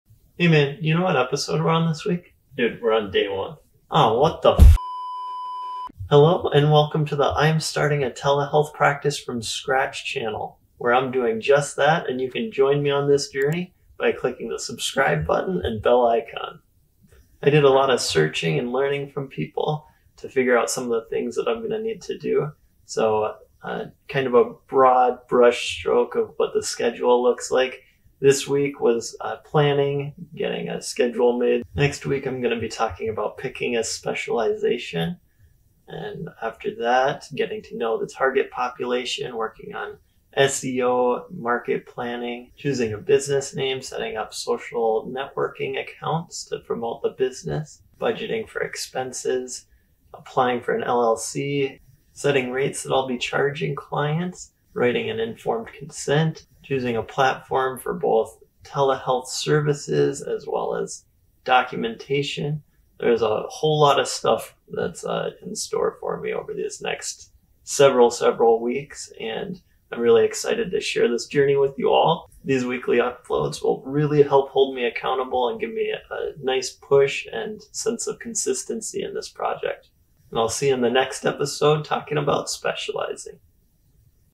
Project Planning and Scheduling | Telehealth Practice
Where do I start when I'm starting a telehealth practice from scratch? This week I decided on project planning and scheduling to continue working towards starting a telehealth private practice. Project scheduling is crucial in ensuring weekly progress towards my long-term goal. My weekly schedule & plan is:

1 Project planning and scheduling
2 Picking a specialization
3 Understanding target population
4 Keyword planning, SEO, & AdWords account
5 Google My Business
6 Choosing a name
7 Creating social media accounts
8 Budgeting for expenses
9 Steps to starting the business
10 LLC
11 NPI
12 EIN
13 Insurance
14 Selecting & setting up a business bank account
15 Setting up HIPAA compliant email & phone
16 Setting a rate for telehealth
17 Select a practice management system
18 Select a book keeping software
19 Prepare informed consent
20 Run "ad campaigns" on FB and google

While I hope to make decent money one day, today is not that day.